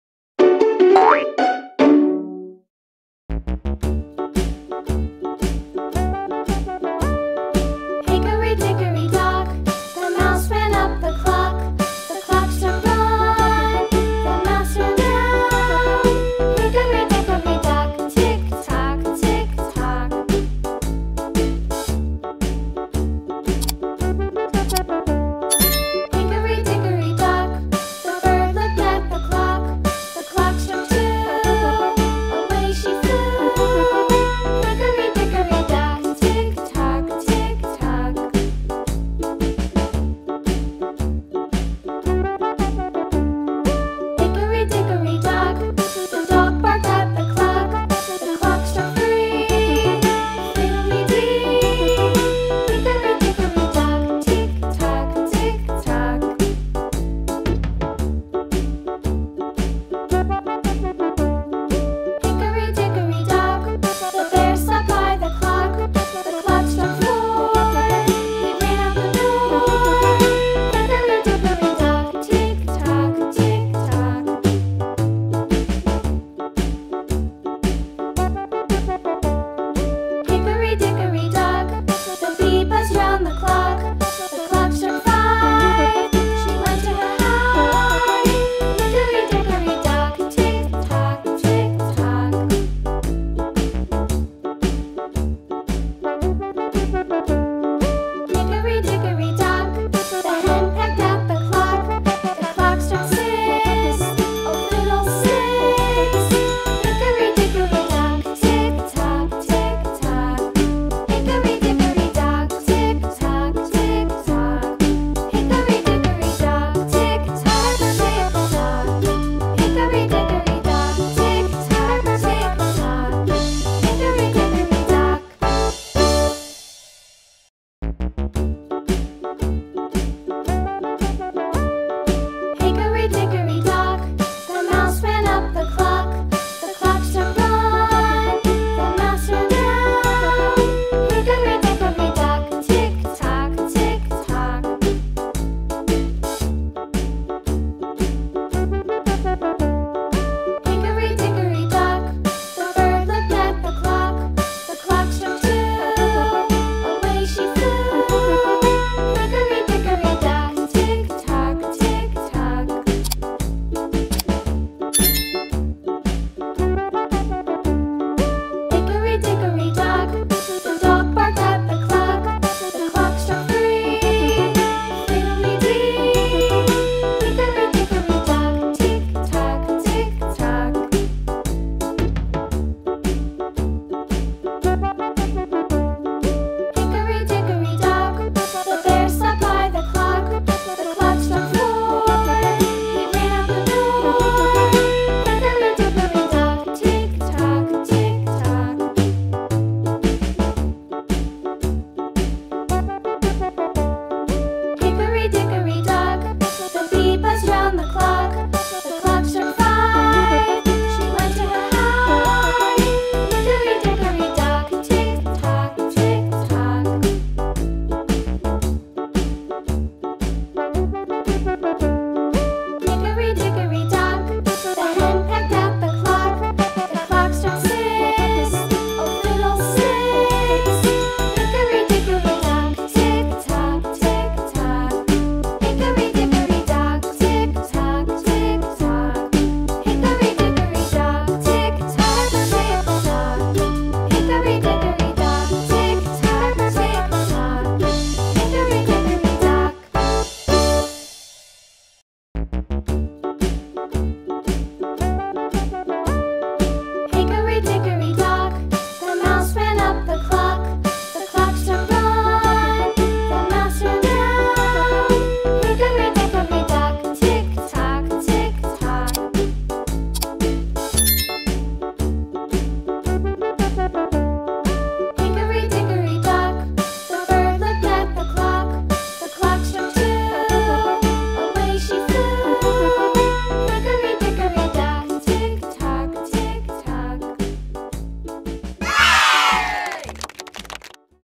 Coloring Peppa Pig 🐷 and Ducks 🦆 | Peppa Feeds the Ducks | Nursery Rhymes for Kids
Let's coloring Peppa Pig  🐷  and Ducks 🦆🦆